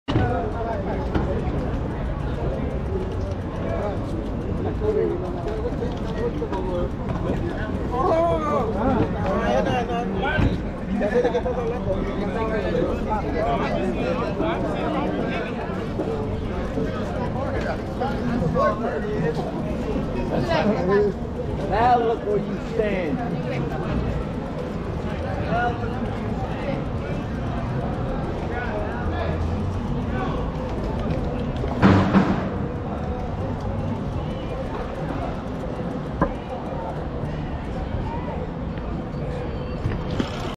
People gather outside the Civic Center BART Main Stairs San Francisco, Market St & 7th St May 2023
People gather outside Civic Center BART Station Market St & 7th St May 2023 7:25 PM